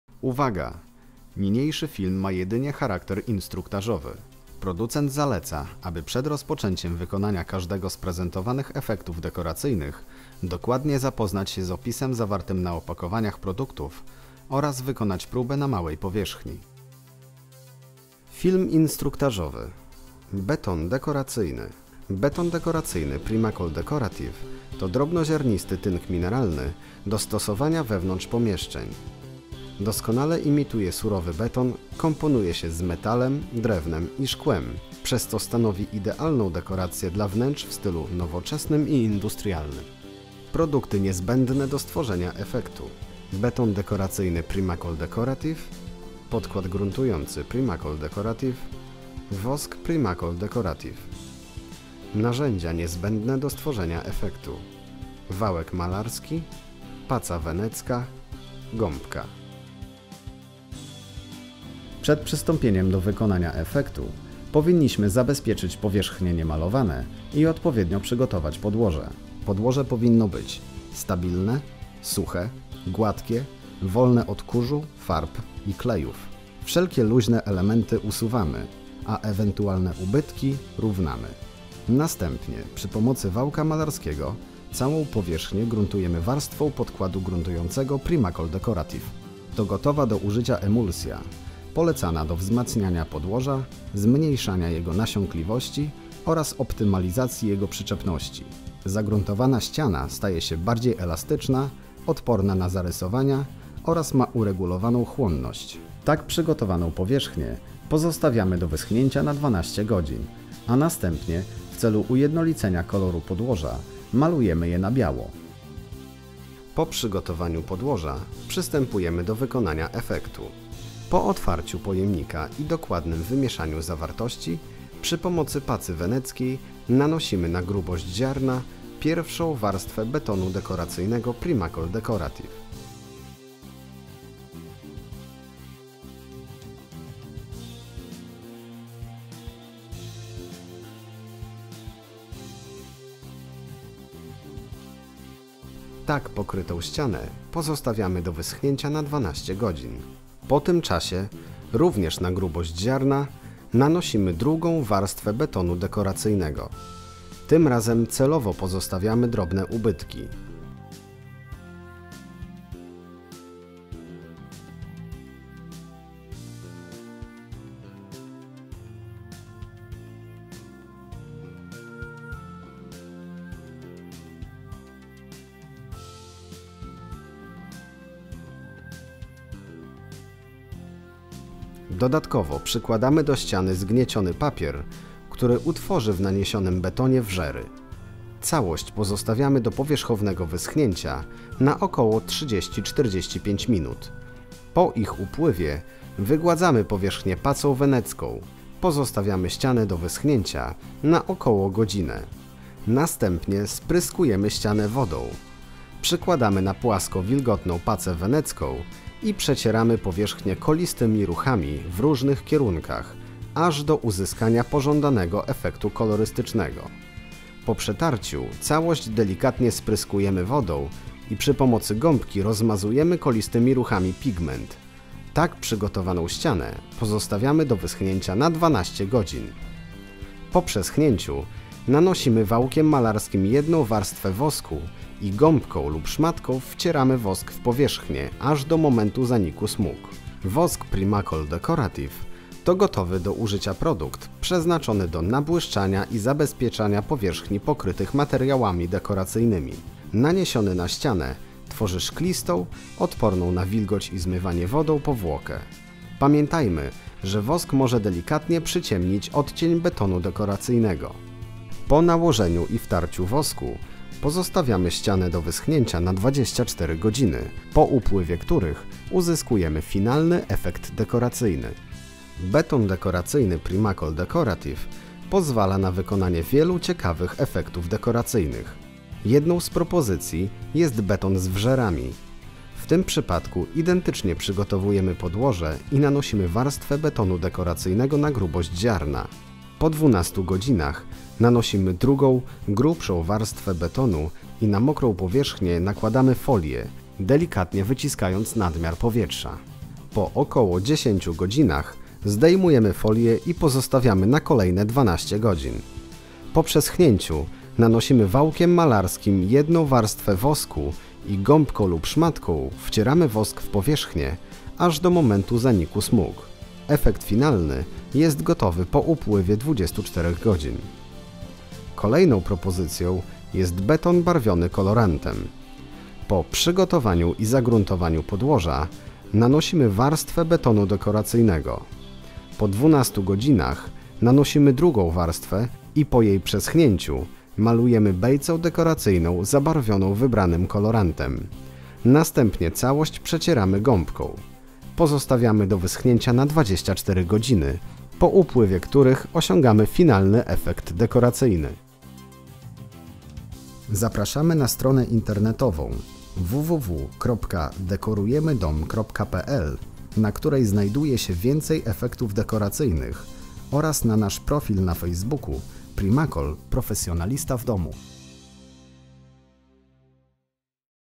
Beton dekoracyjny Primacol Decorative - film instruktażowy
Zobacz, jakie możliwości w dekorowaniu wnętrz daje Beton dekoracyjny marki Primacol Decorative. Prezentujemy, jak krok po kroku wykonać dekorację ścian wykorzystując Beton dekoracyjny i inne produkty z Systemu Kreatywnej Dekoracji Primacol Decorative.
Beton dekoracyjny Primacol Decorative to drobnoziarnisty tynk mineralny do stosowania wewnątrz pomieszczeń. Doskonale imituje surowy beton, komponuje się z metalem drewnem i szkłem, przez co stanowi idealną dekorację dla wnętrz w stylu nowoczesnym i industrialnym.